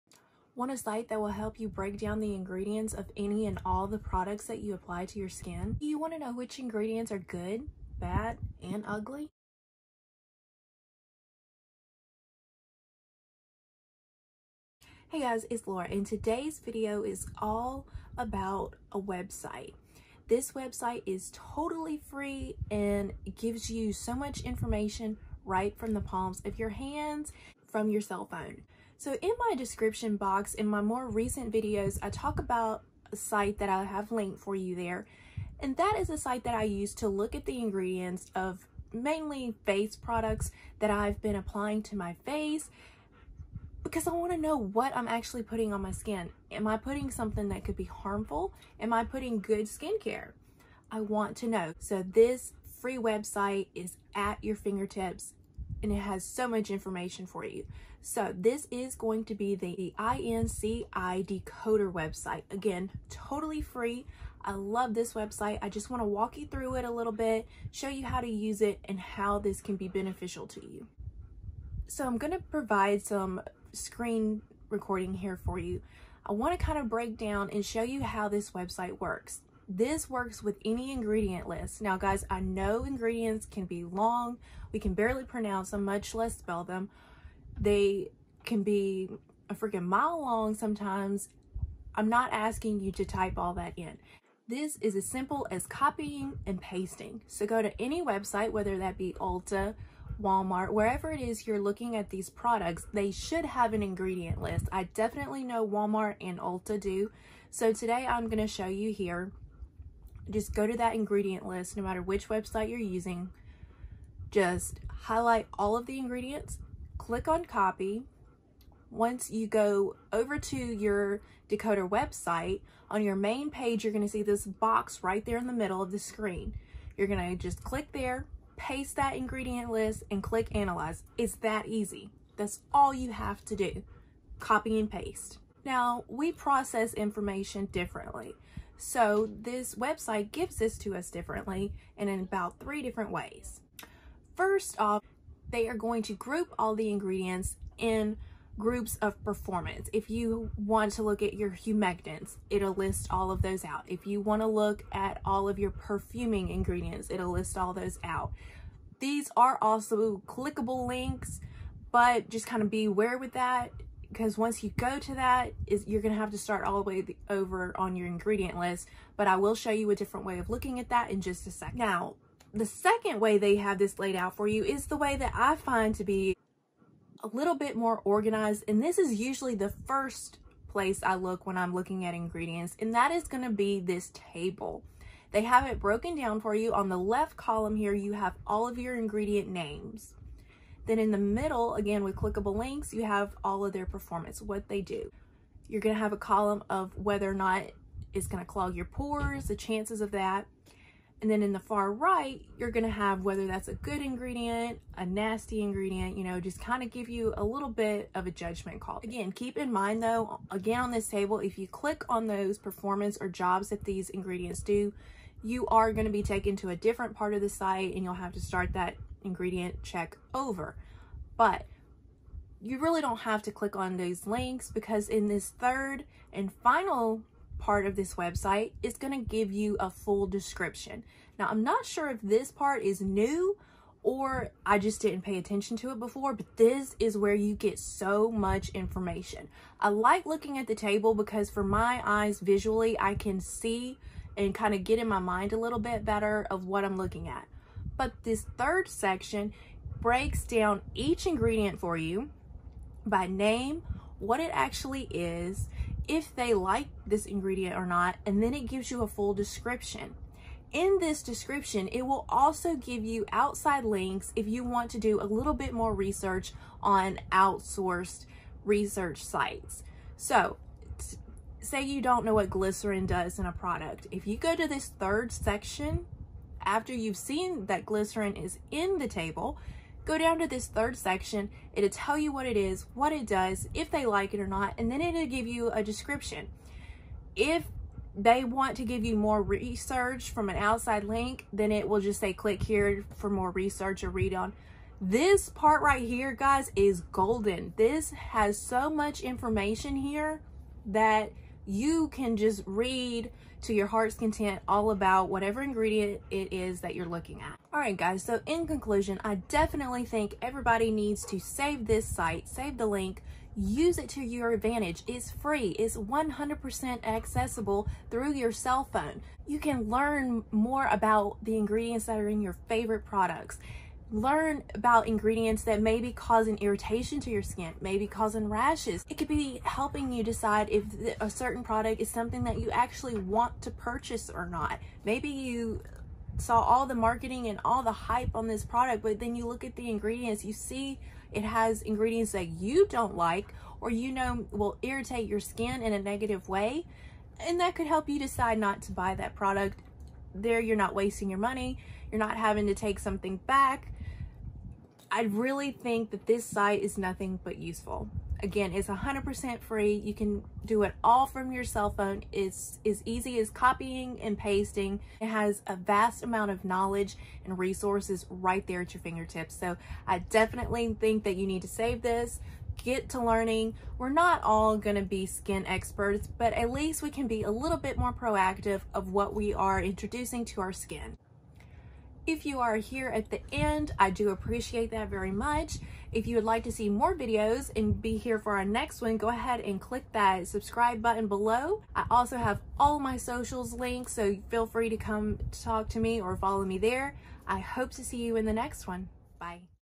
How to Use the INCI Decoder Site -Laura Garza
Today’s video is on the INCI Decoder site. This site will analyze the ingredients of almost anything. I mainly use it to look at the ingredients of my skincare and makeup products. It’s 100% free and easy to use.

**Research Site:
INCIDecoder:

garza_girl1

garza_girl1

Garza Girl 1